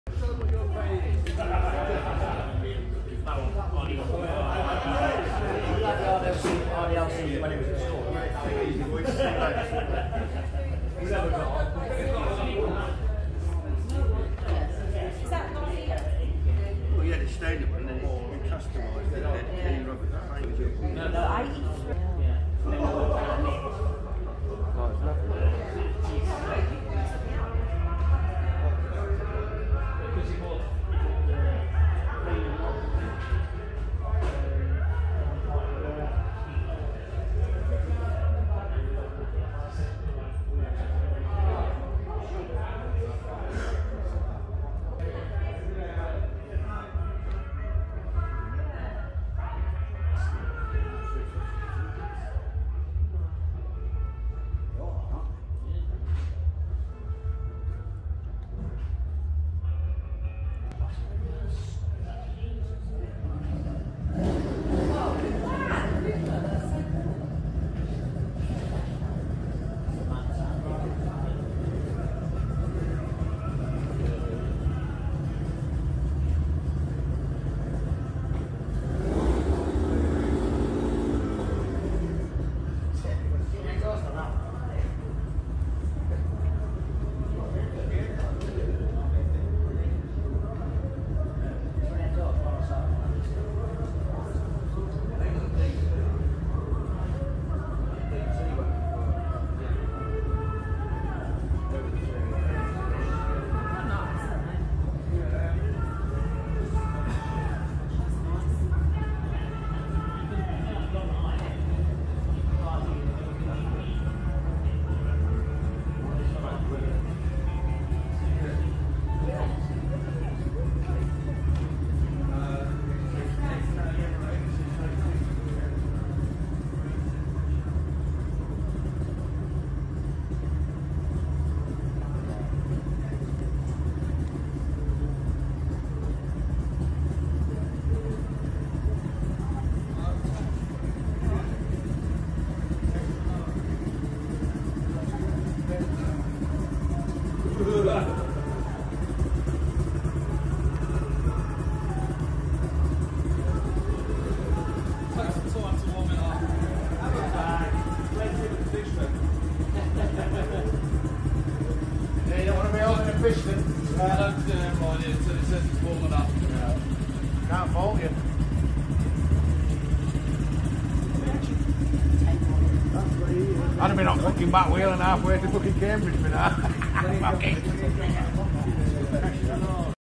Mouth Watering 2 Strokes on Display at MCN Festival
Some of the coolest 2-stroke classic bikes have been displayed at MCN festival.
Yamaha’s beloved RD LC was forty years old in 2020 and MCN has created an extra special belated birthday party at the Festival with a two-stroke display, fire ups and stage talks.